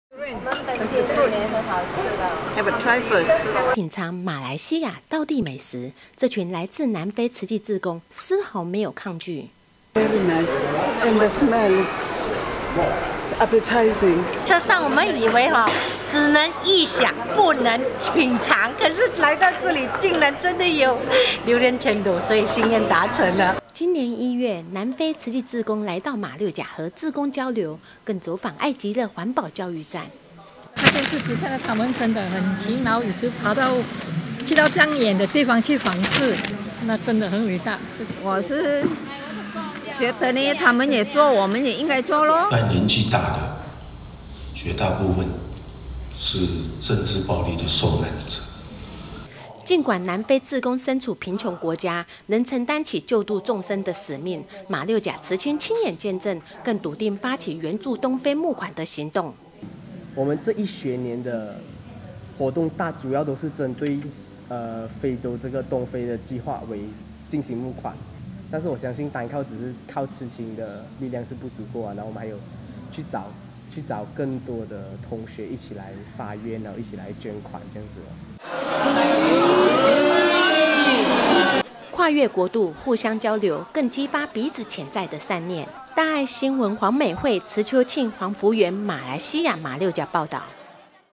以爱传爱不停歇 善念循环不打烊[HD]
一连两天，南非慈济志工走访马六甲分会，与志工见证分享与交流，相互学习。

本地榴莲很好吃的，试试看
品尝马来西亚道地美食，这群来自南非慈济志工丝毫没有抗拒。
南非慈济志工 杜杜恩勾伯：很好吃，它的味道很浓郁。
雪隆慈济志工 叶妤稹：在车上我们以为只能意想不能尝到，可是来到这里竟然有榴莲晶露，所以（他们）心愿达成了。

今年1月，南非慈济志工来到马六甲和志工交流，更走访爱极乐环保教育站。
环保志工 梁华凤：在电视机看到他们真的很很勤劳，有时去到那么远的地方去访视。
环保志工 毛金贤：我是觉得他们也做，我们也应该做。

年纪大的，绝大部分政治暴力的受难者。
尽管南非志工身处贫困国家，仍承担起救渡众生的使命，马六甲慈青亲眼见证，更笃定发起援助东非募款的行动。
慈青 张国浩：我们这一学年的活动主要是针对非洲东非这个计划进行募款，但是我相信单靠慈青的力量不足够，我们还有去找更多的同学一起来发愿，一起来捐款。

慈济
跨越国度，互相交流，更激发彼此潜在的善念。

大爱新闻 黄美慧 池秋庆 黄福源 马来西亚马六甲报导